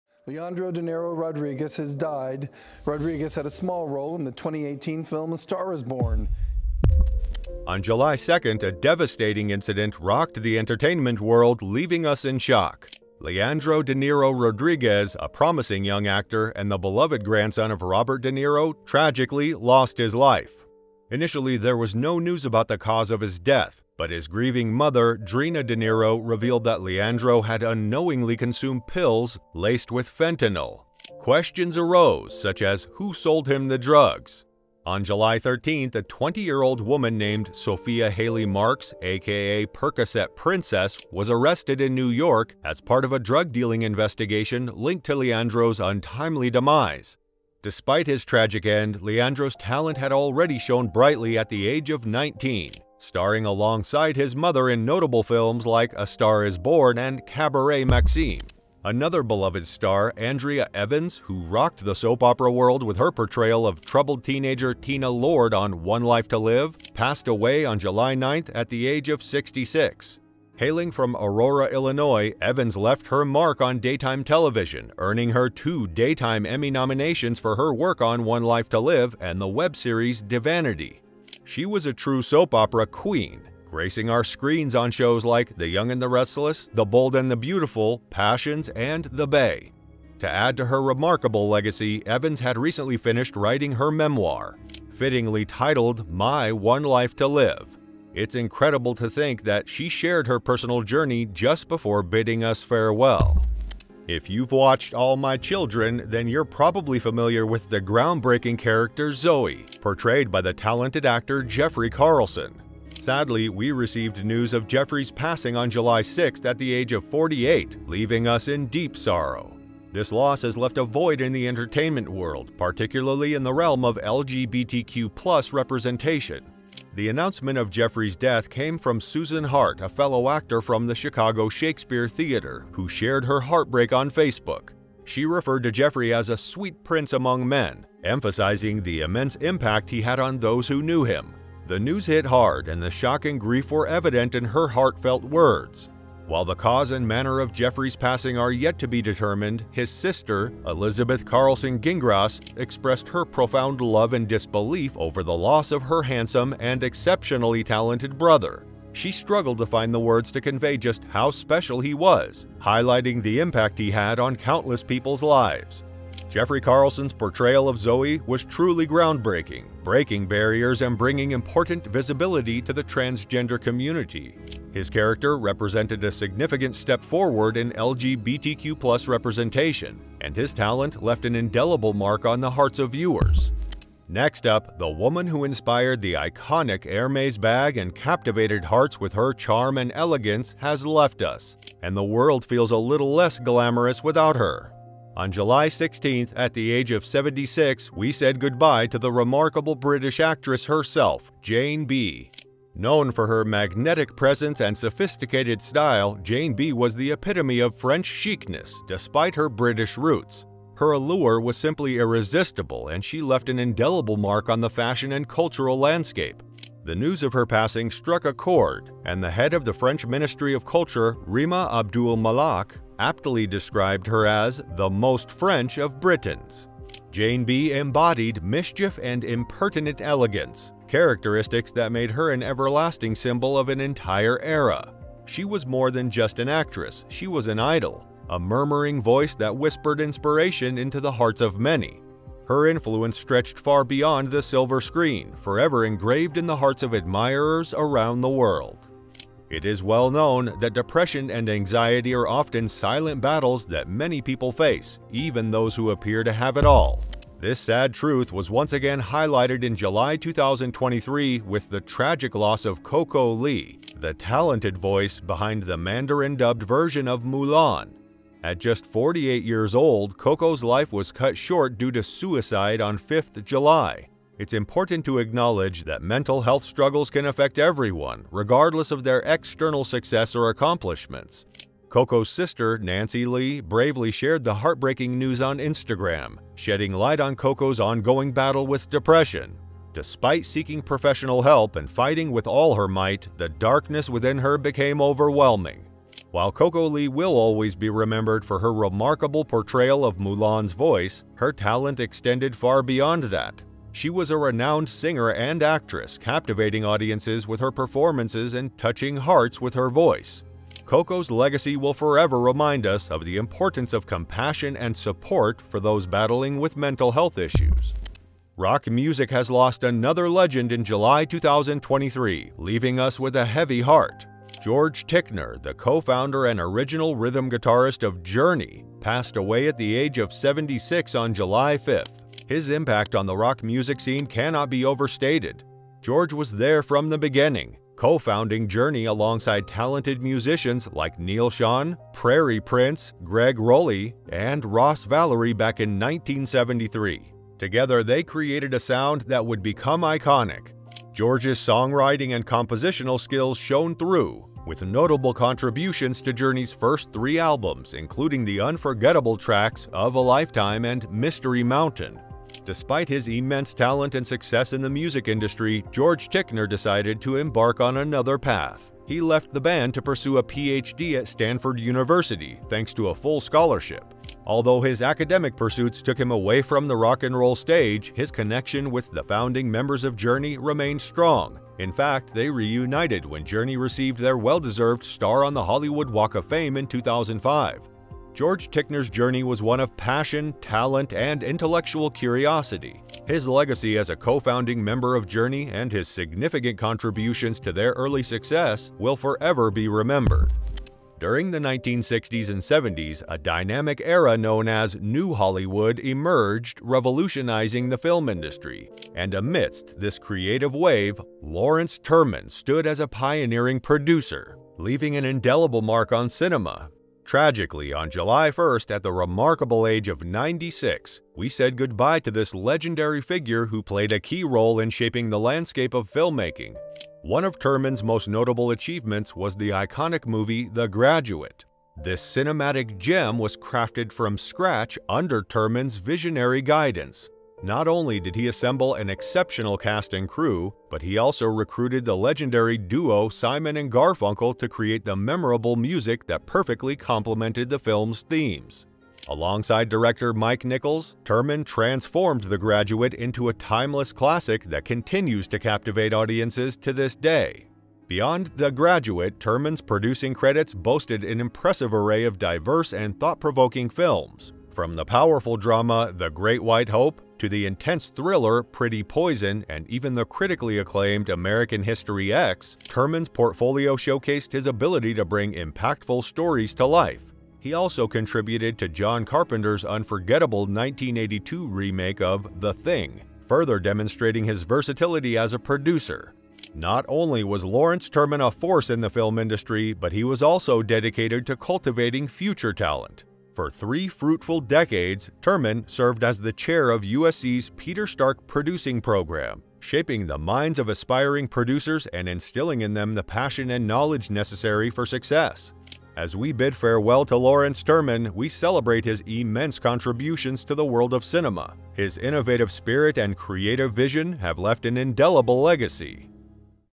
Celebrities Who Died In July 2023
Breaking news: Celebrities who died recently
Leandro De Niro-Rodriguez - Andrea Evans - Jeffrey Carlson - Jane Birkin - CoCo Lee - George Tickner - Lawrence Turman

Can you believe how unpredictable life can be? In the past weeks, we were hit with a wave of loss as some of our favorite stars bid us farewell. From legendary musicians who rocked our world to TV icons who brightened our screens, and incredible actors who brought characters to life, they left a void that can never be filled. Here, we'll explore the celebrities we lost in 2023, and trust me, there are some surprises you won't even see coming